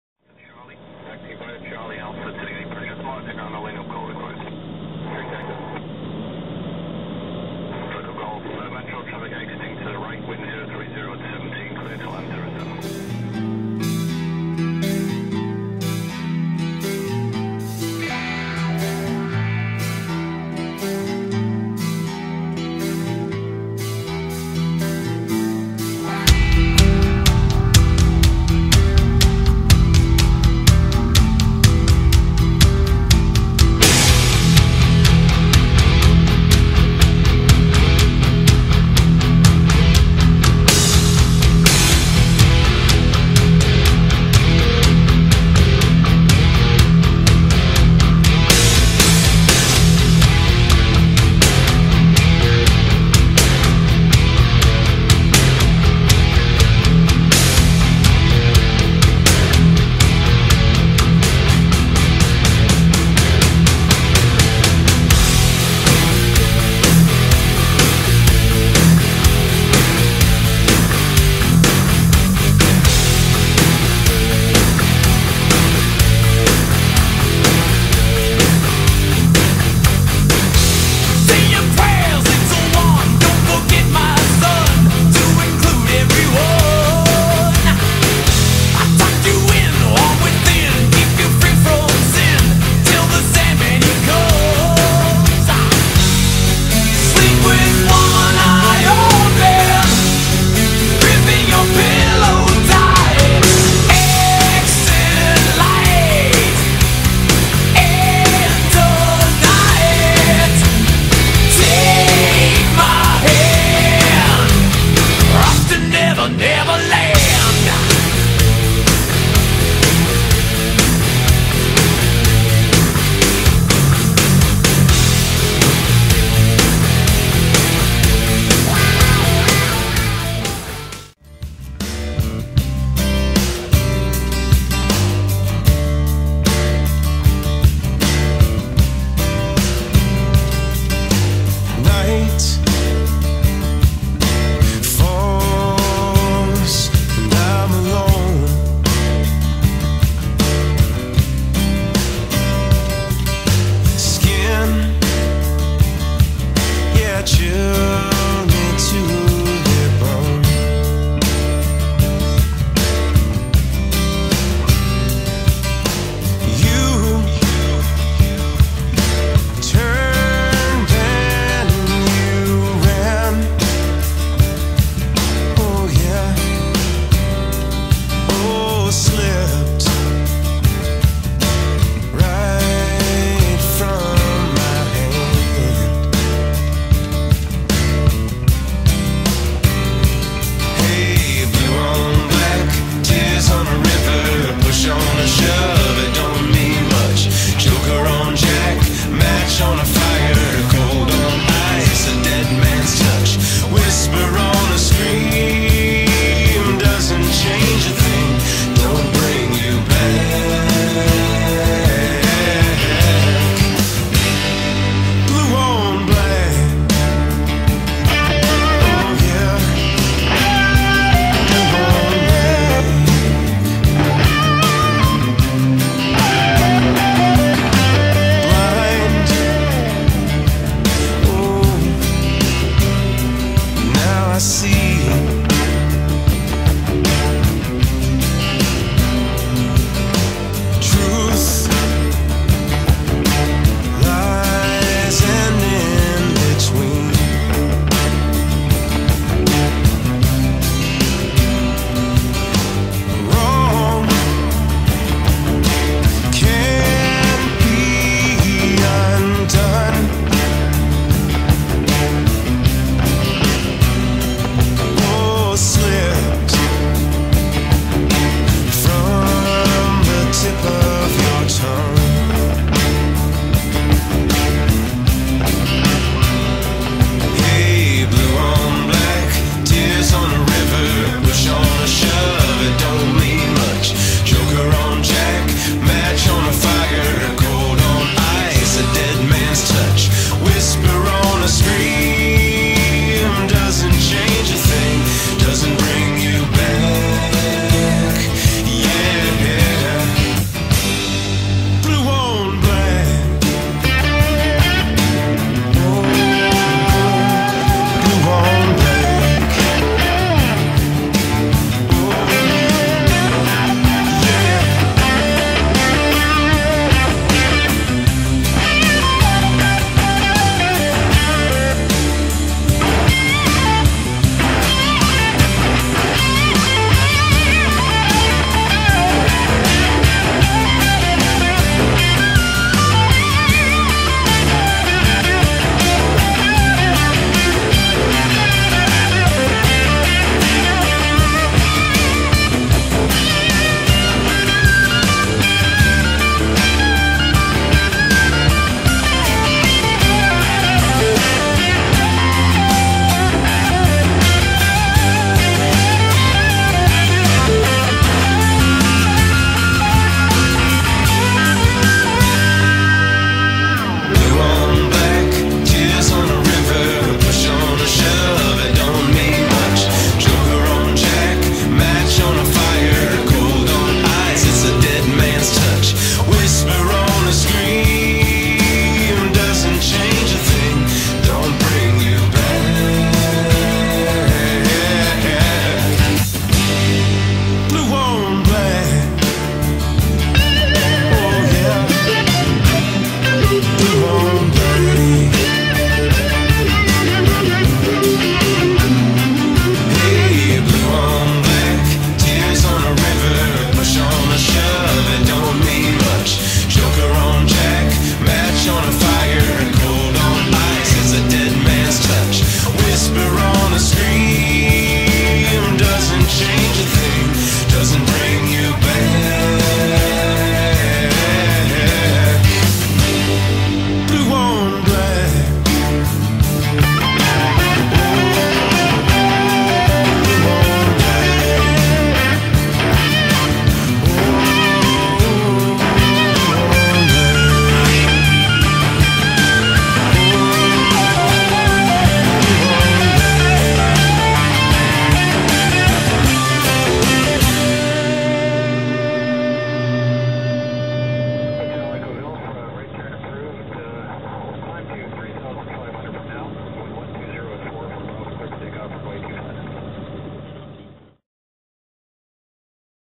Northern Ontario Flying Vids 2
Some more random flying videos from my time at Slate Falls Airways